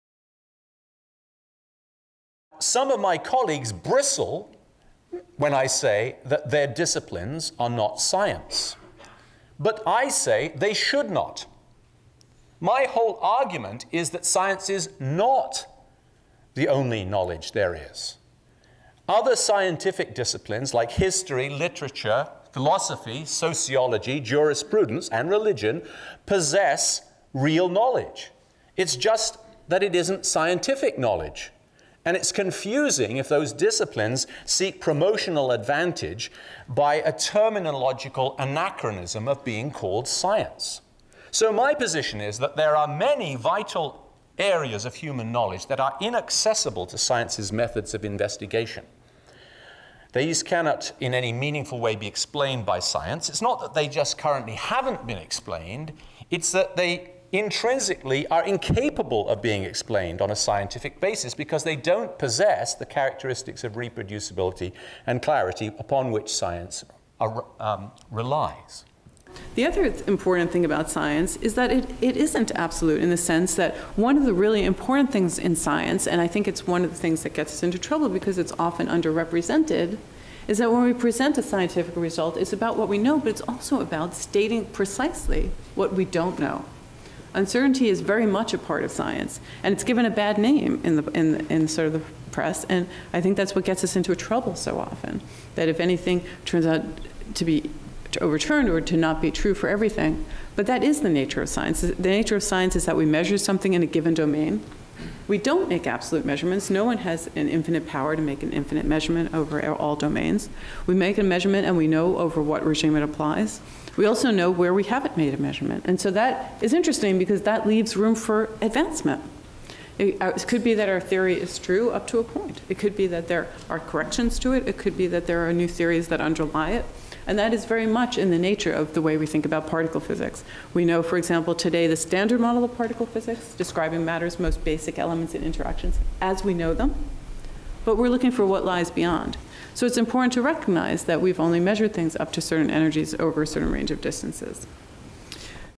Can Science Explain Everything?
On December 6, 2011 the AAAS Dialogue on Science, Ethics and Religion (DoSER) held an event called "Can Science Explain Everything?" in which MIT physcist Dr. Ian Hutchinson and Harvard physicist Dr. Lisa Randall discuss their thoughts on the limits and powers of science. To watch the entire event and read more about this fascinating subject please